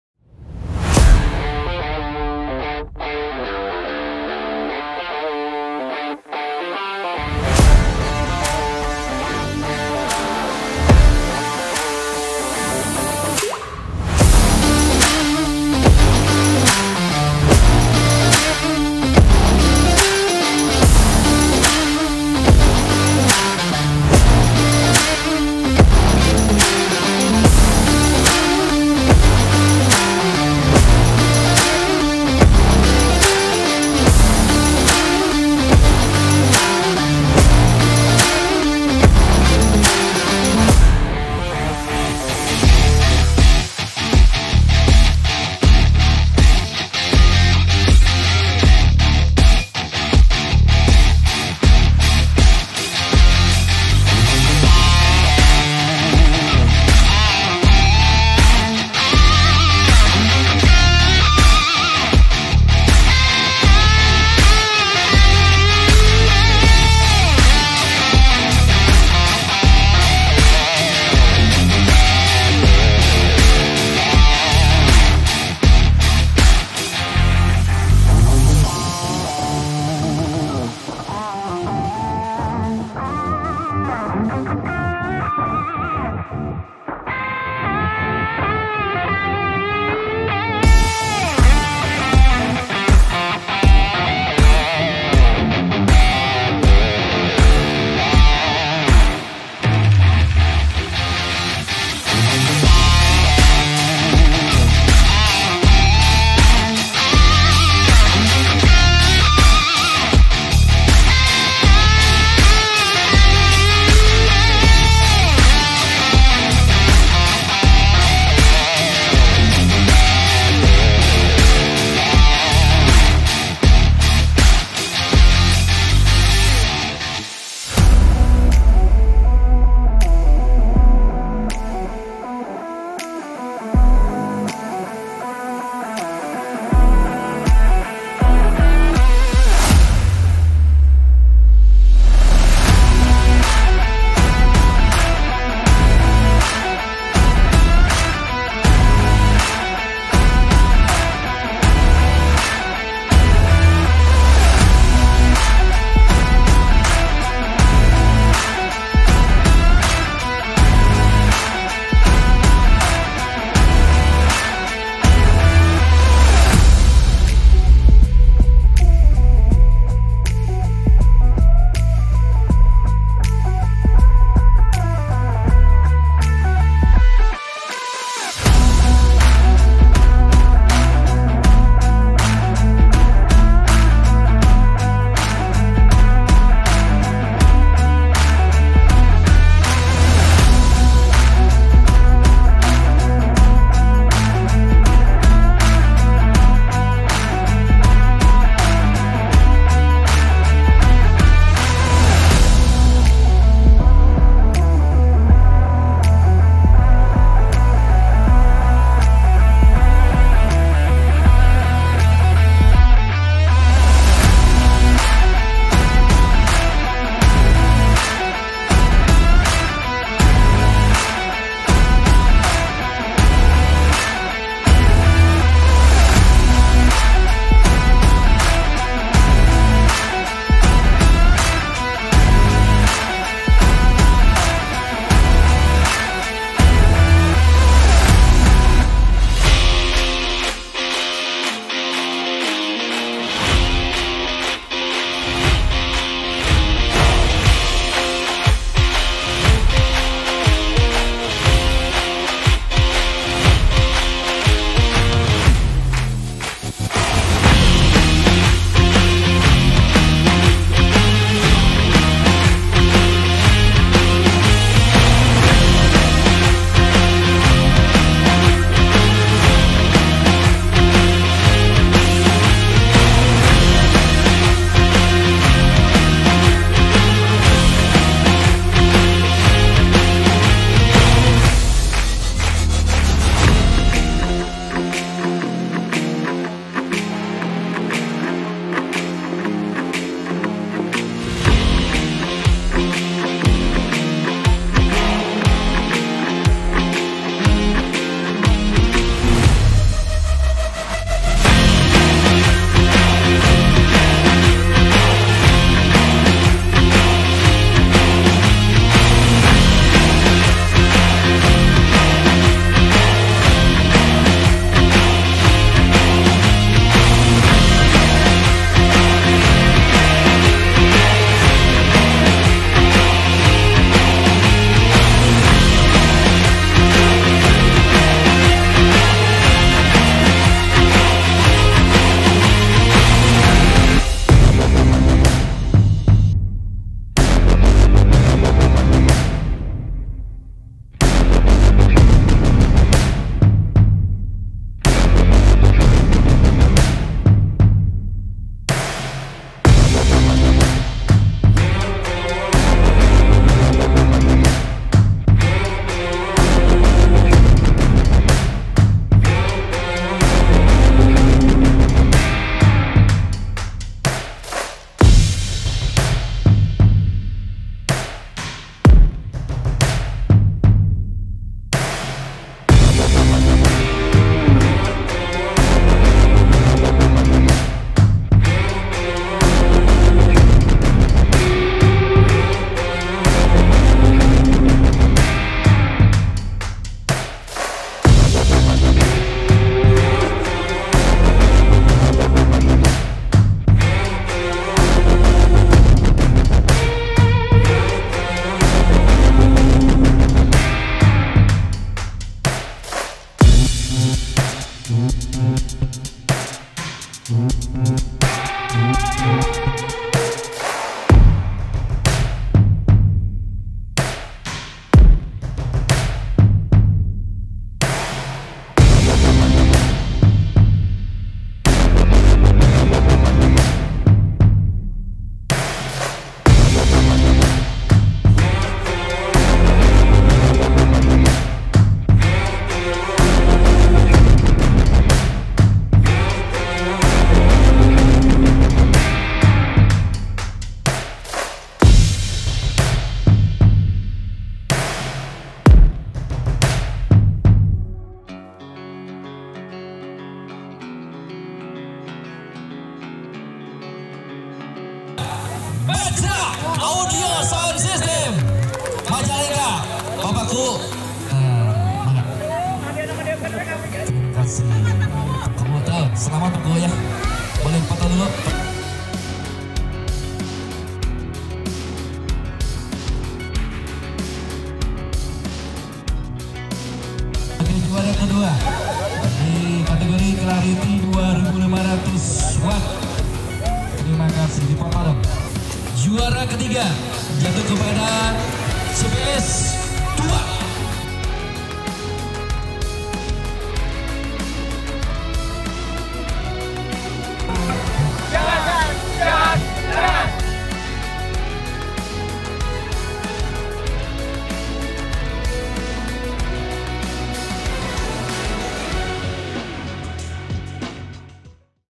CLARITY LIVE MIXING 2500-5000 WATT | ACARA SILATURAHMI NYETEL BARENG Drs H. YOD MINTARAGA, M.PA CUP
ACARA SILATURAHMI NYETEL BARENG SOUND SYSTEM
Drs H. YOD MINTARAGA, M.PA